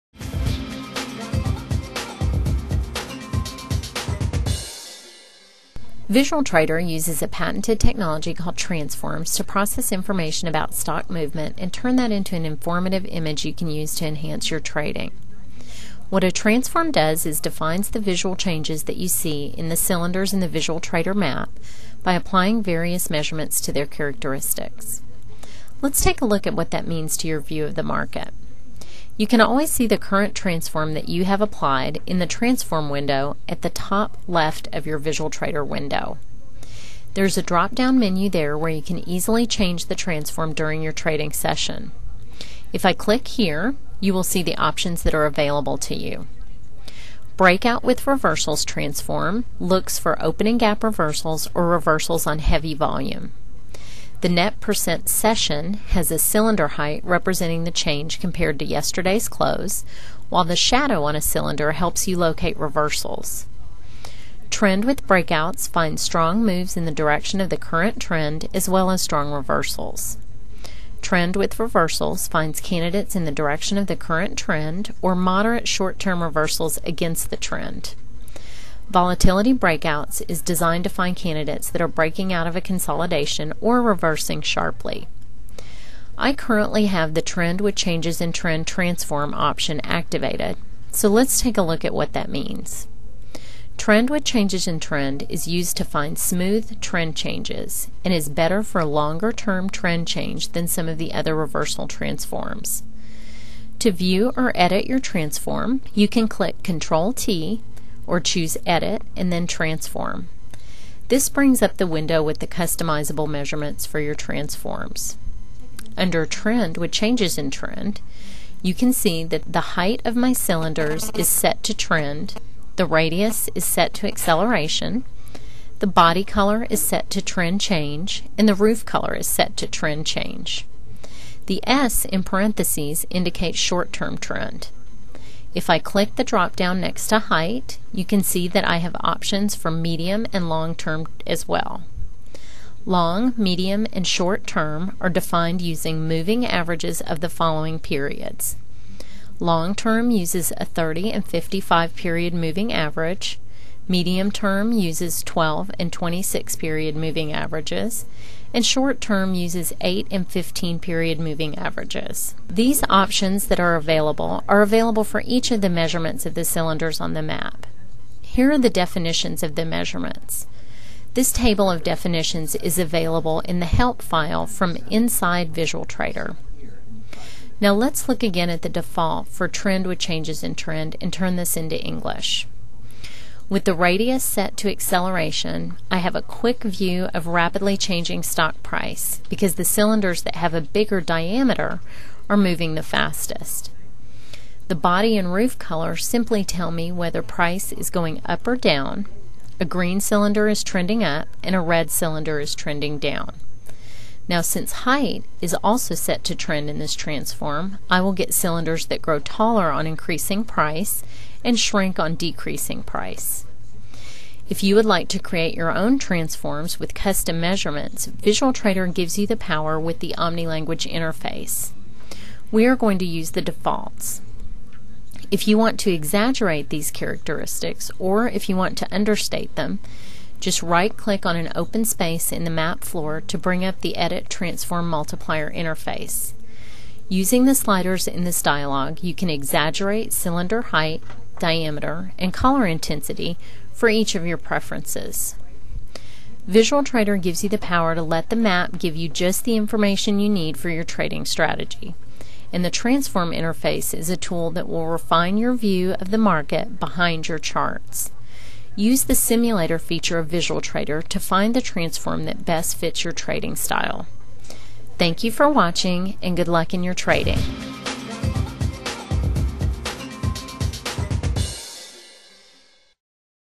VisualTrader Lesson of the week - Transforms
Learn how to use Transforms in VisualTrader.